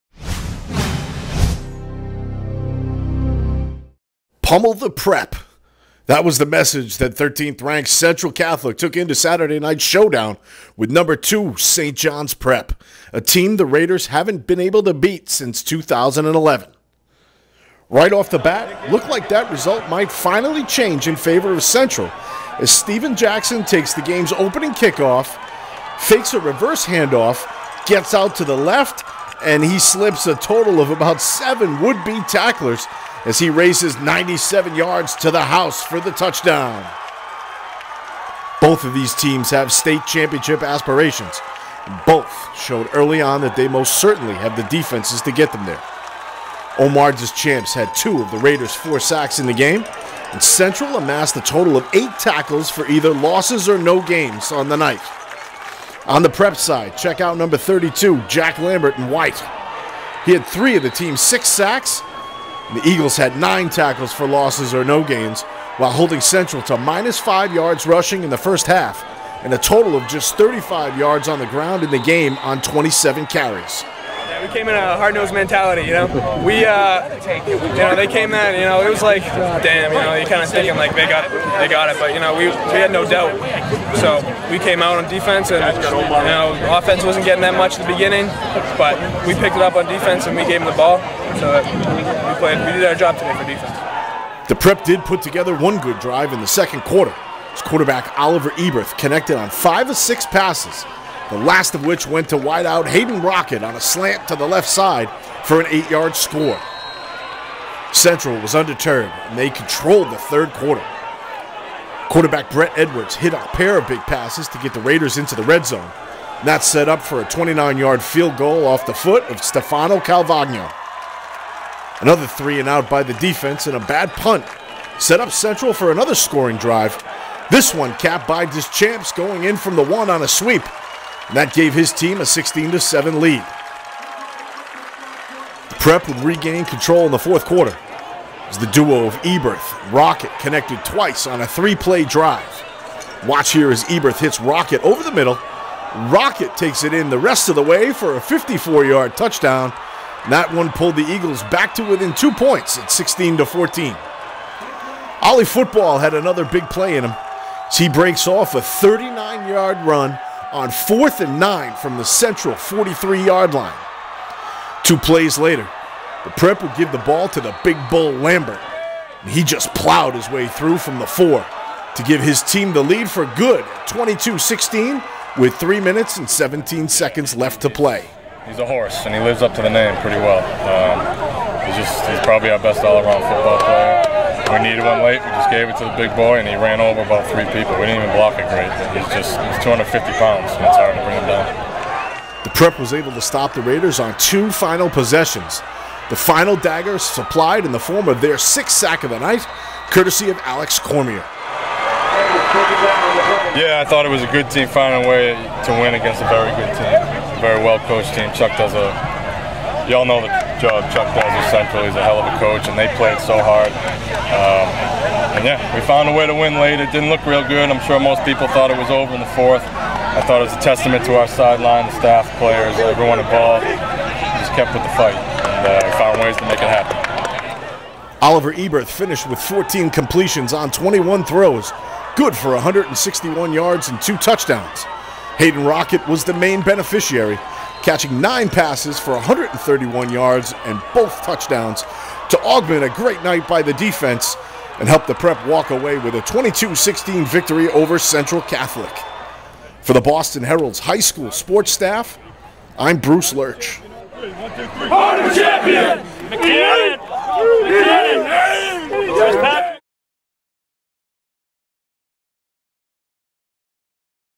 High School Football: No. 2 St. John's Prep 22, No. 13 Central Catholic 16
Video highlights from No. 2 St. John's Prep's come-from-behind, 22-16 victory over No. 13 Central Catholic. Led by quarterback Oliver Eberth, the Eagles scored twice in the fourth quarter to rally from a 16-7 deficit.